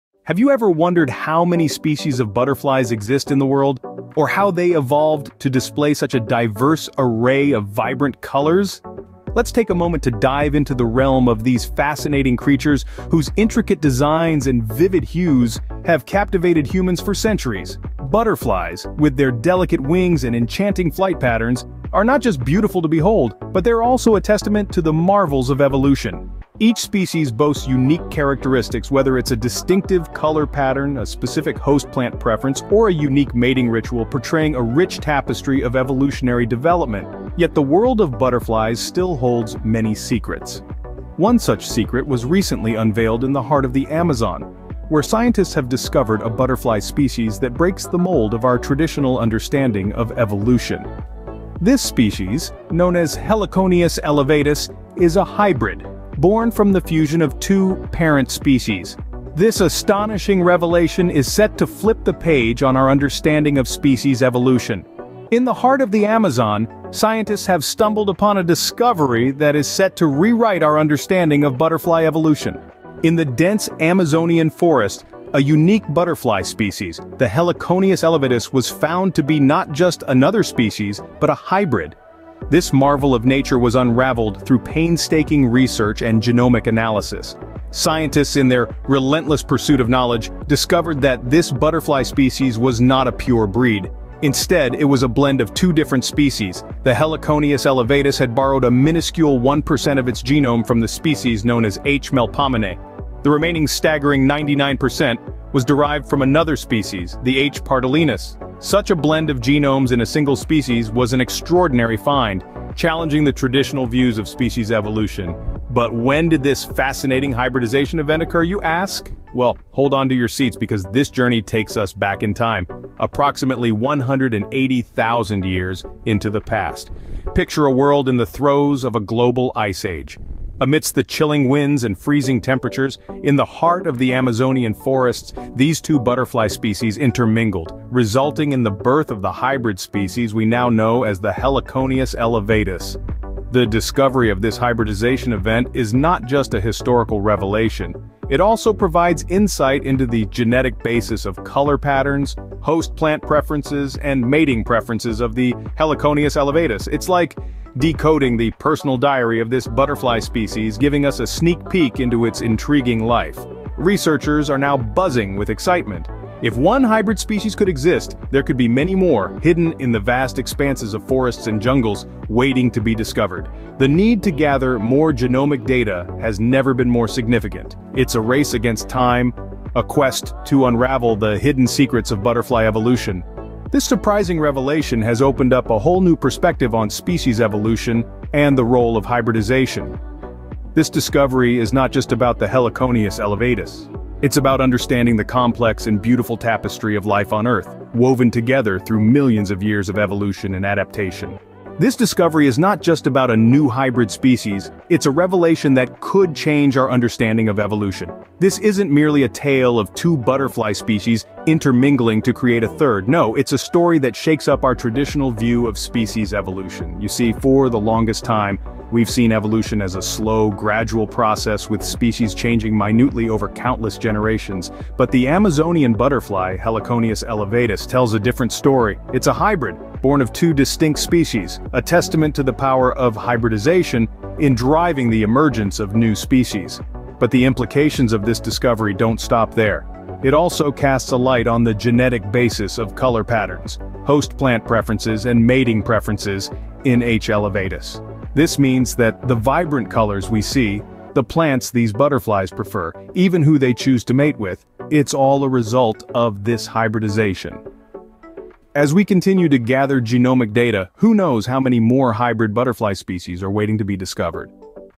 The Mysterious Depths Of The Hybrid Butterfly (Documentary)
Dive into the enchanting world of the Hybrid Butterfly in this captivating documentary. Explore the mysterious depths of butterfly behavior, conservation efforts, identification, and the importance of pollination. Witness the vibrant colors, majestic wings, and fluttering beauty of these winged creatures as you learn about their migration patterns, population, and unique species. Join us on a journey to discover the wonders of these colorful insects and their vital role in wildlife conservation. Capture the essence of these winged wonders through butterfly photography and marvel at the intricate wing patterns of the monarch butterfly. Immerse yourself in the mesmerizing world of these fluttering insects and appreciate the beauty they bring to our environment.